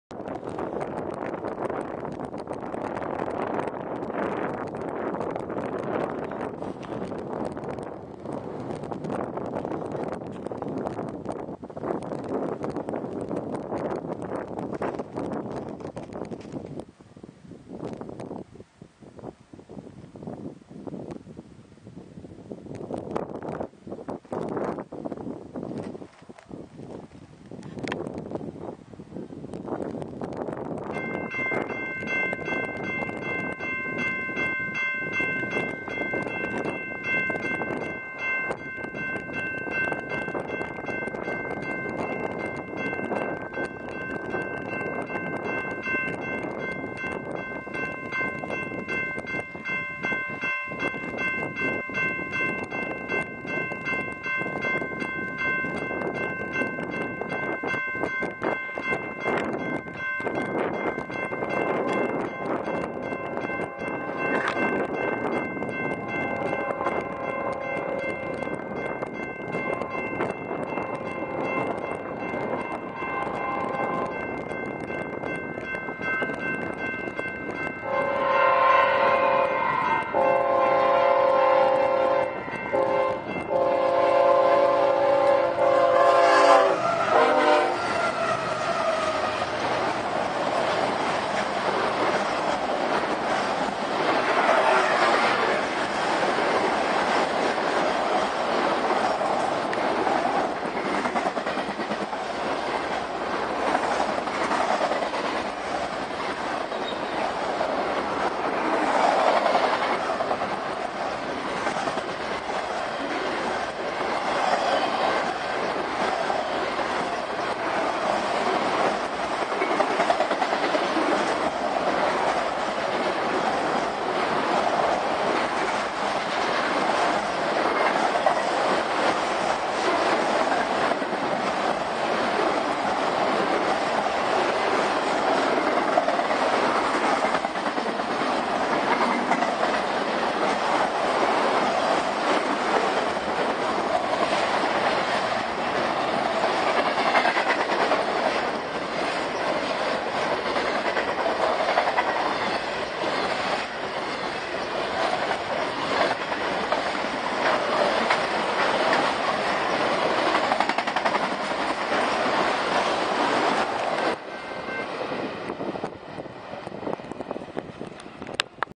BNSF through Escalon
Railfanning the back country of Escalon away from the automatic horns in downtown saw four trains 2 southbound trains and 2 northbound.Though this was by far my best video ever. The crew was very nice and put on a show for my camera. The cars in the autoracks where filled with Fiat 500's and Toyota prius. Special thanks to my dad who drove me to Escalon